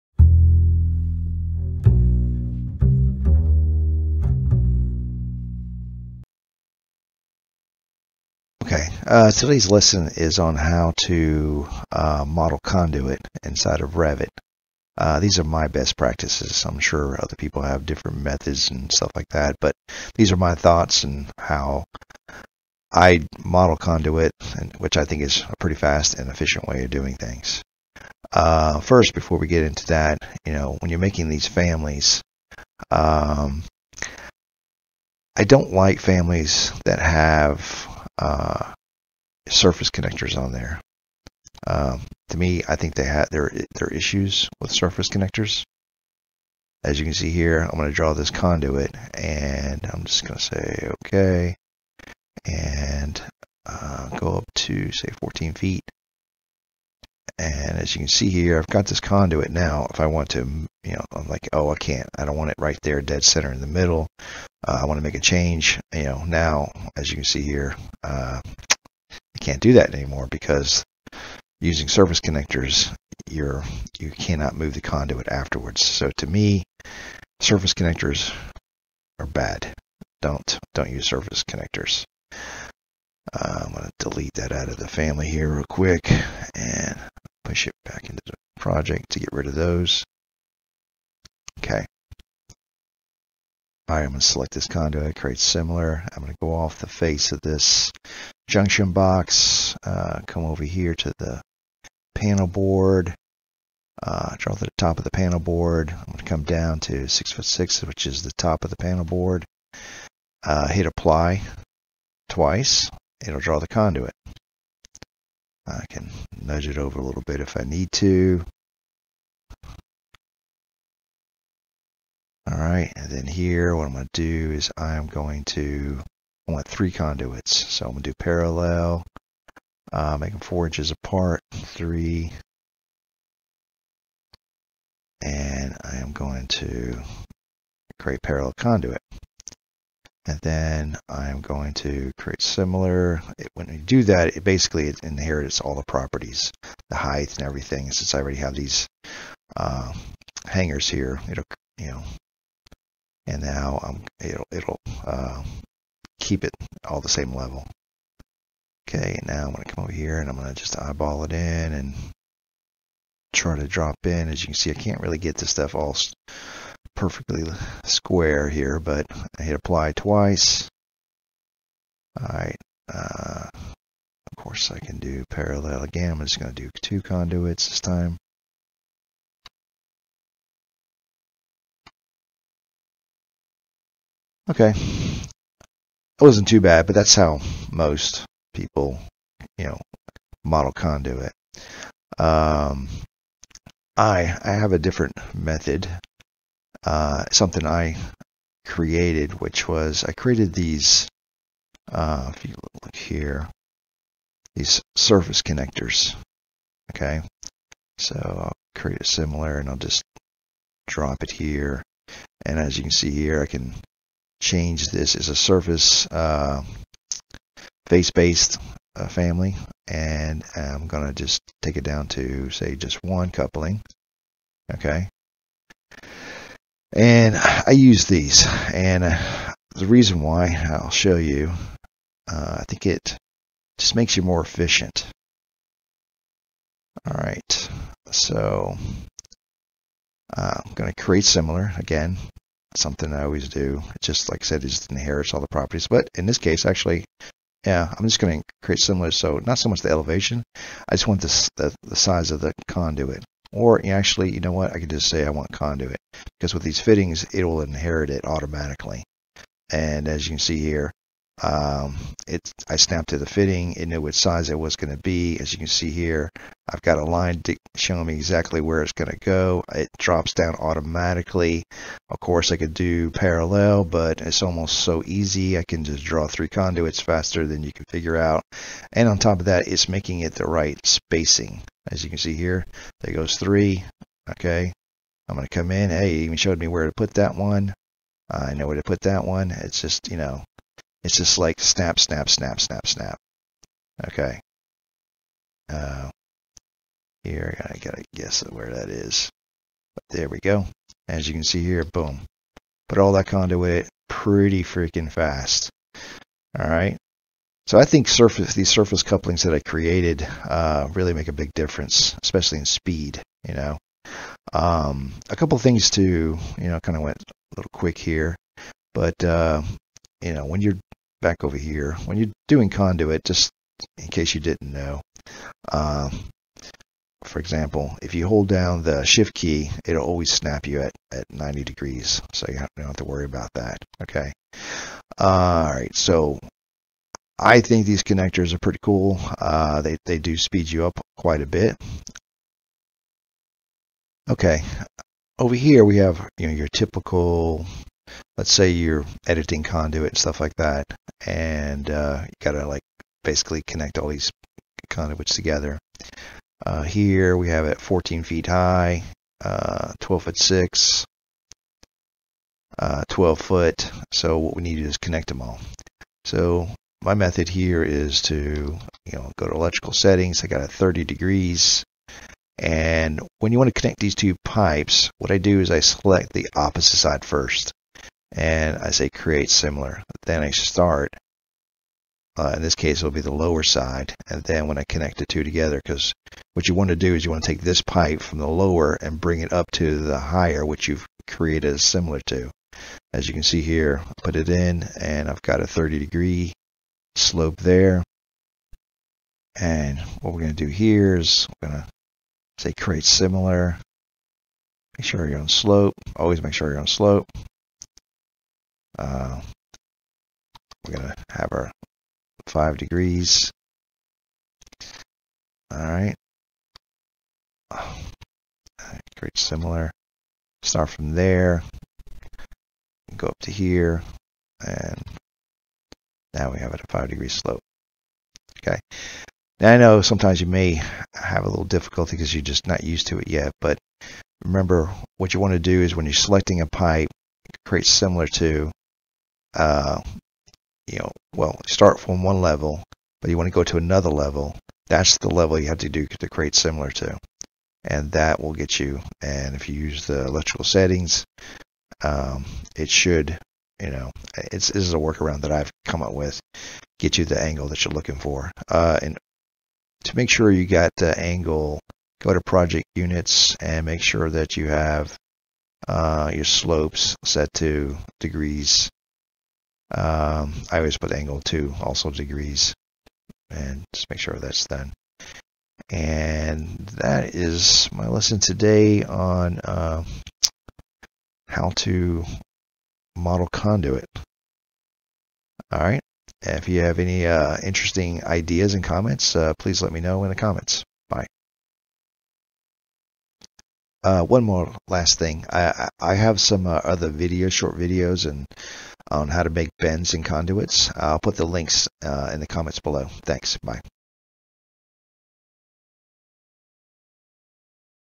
Revit Electrical - Modeling Conduit Tutorial in 3D views
In this video, we breakdown the entire workflow of modeling and editing Conduits in Revit.

Time Stamps
0:00 - Modeling conduit with Surface Connectors
1:32 - Modeling conduit traditional way
3:16 - Modeling conduit alternate method
5:43 - Modify conduit runs
5:13 - Modeling conduit Rolling Offset in Revit

If you find this lesson useful and learned something new!